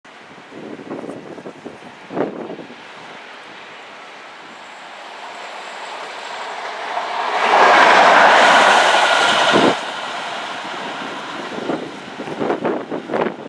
HO FILMATO ANCHE QUI!! ED IL REG VELOCE TIRA!!!!
REG 22753 Catanzaro Lido - Reggio Cal. C.le in transito nelle zone di Pellaro.